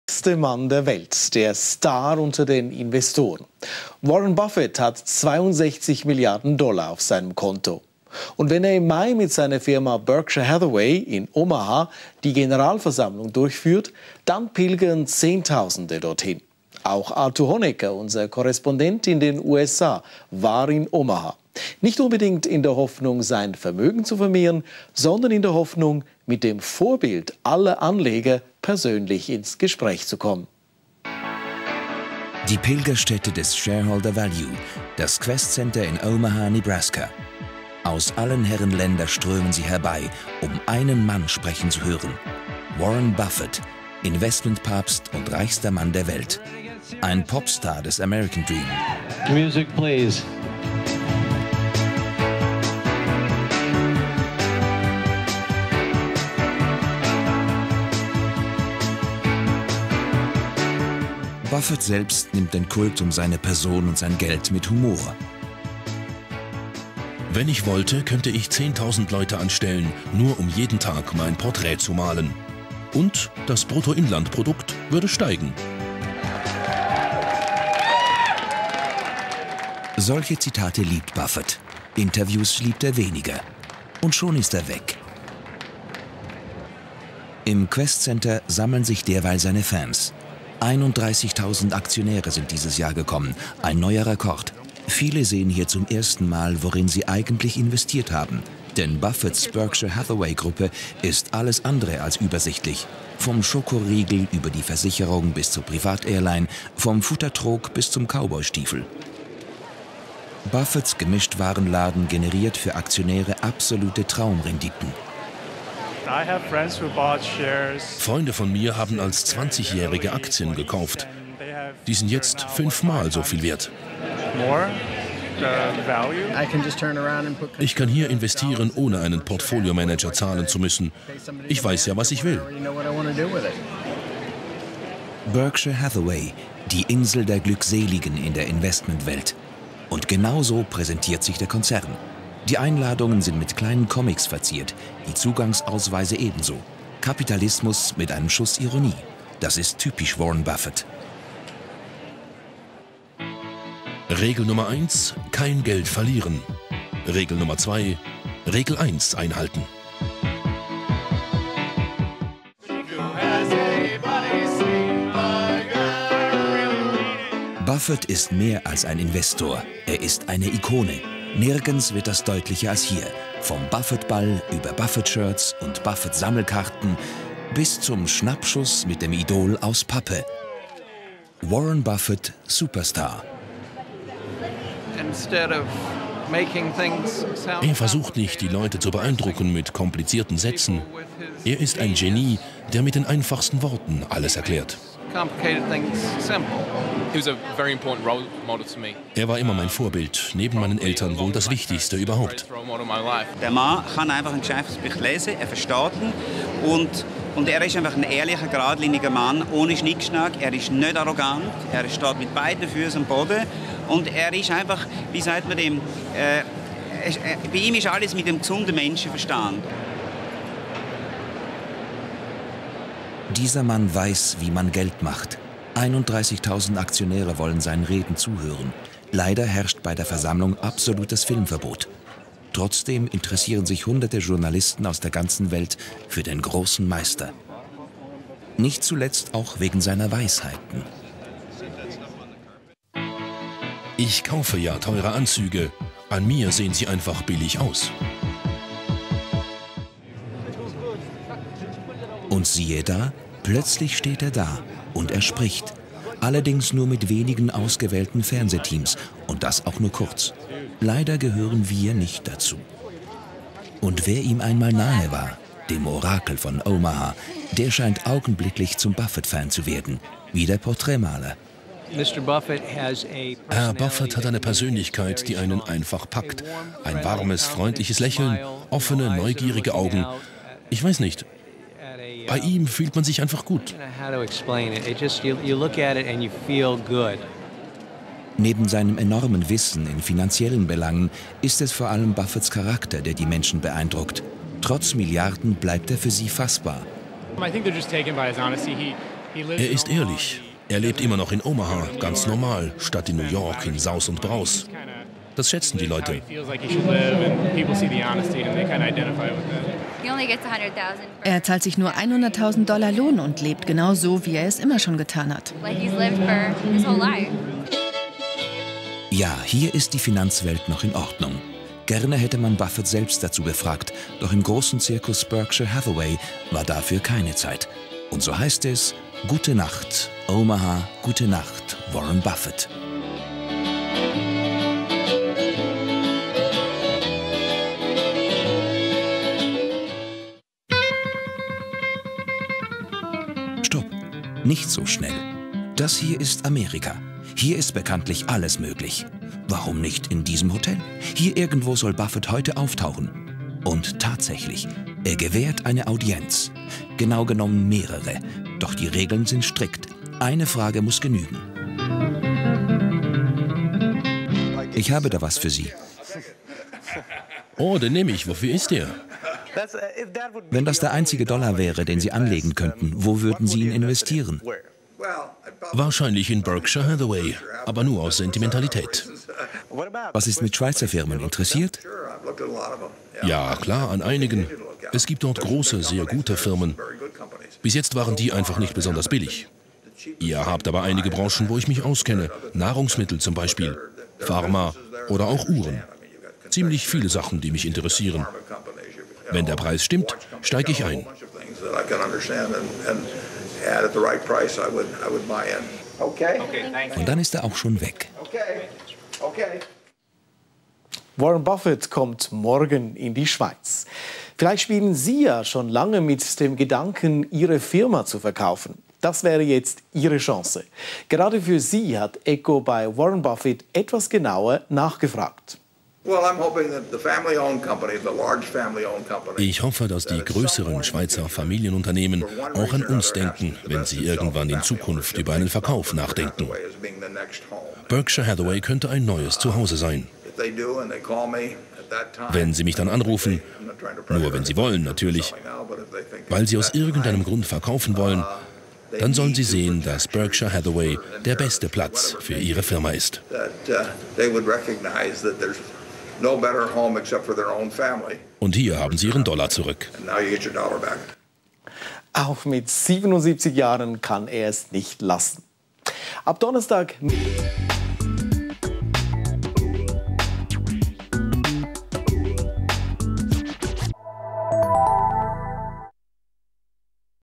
ECO - Interview mit Warren Buffet
Arthur Honegger hat den reichsten Mann der Welt getroffen --